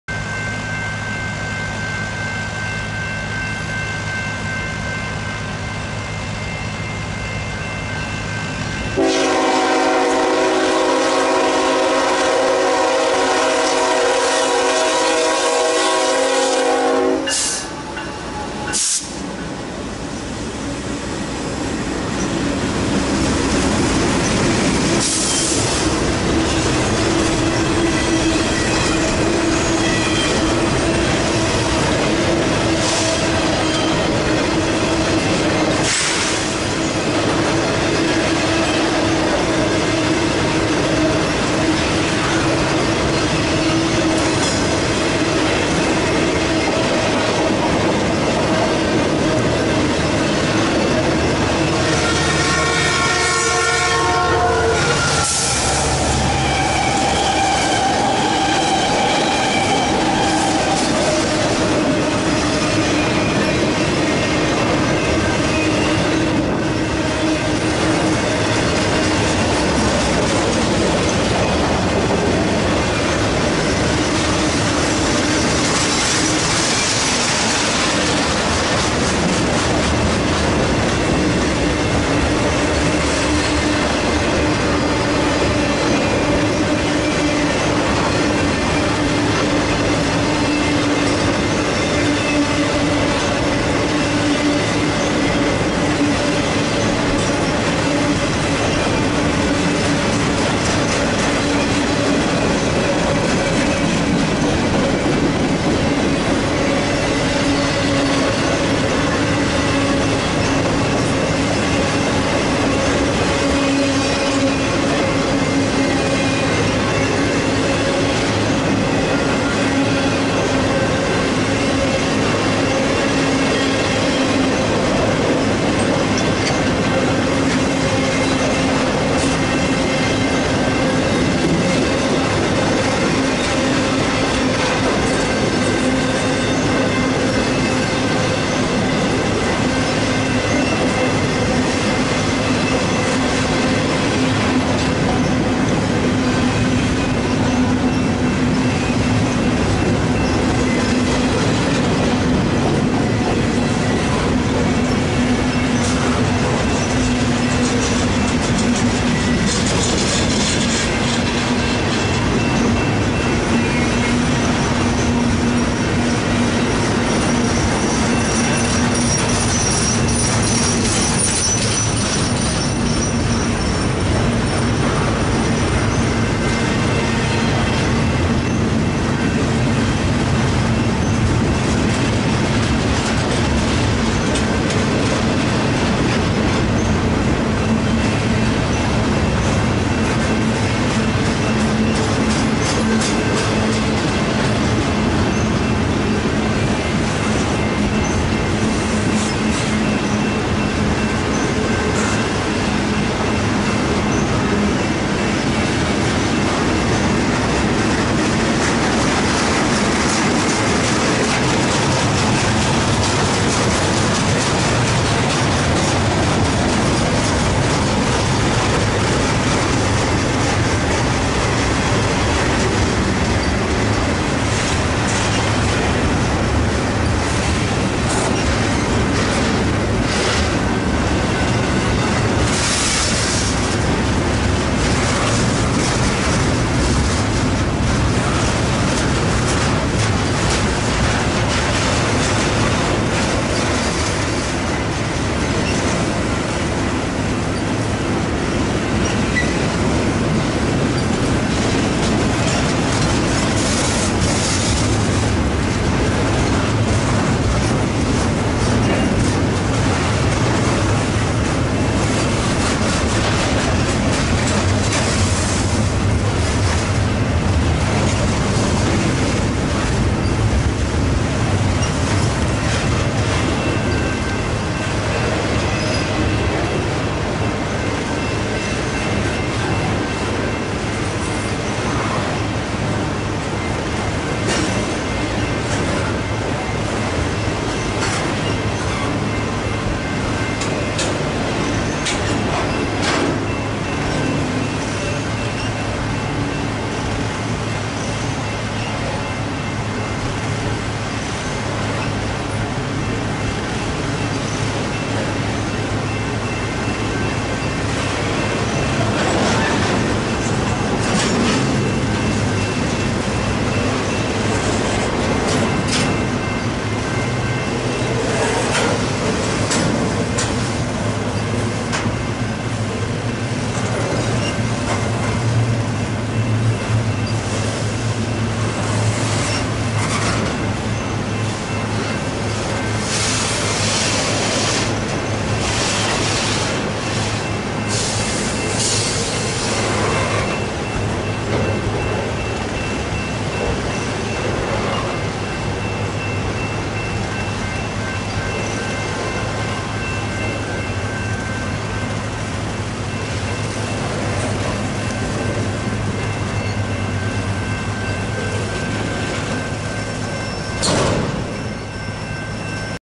BNSF ET44C4 Intermodal with Loud Horn stopping at Santa Fe Springs
1 BNSF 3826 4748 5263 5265 leads an east bound intermodal into Santa Fe Springs.
2 SCAX 880 F59PHI Metrolink sneaking by.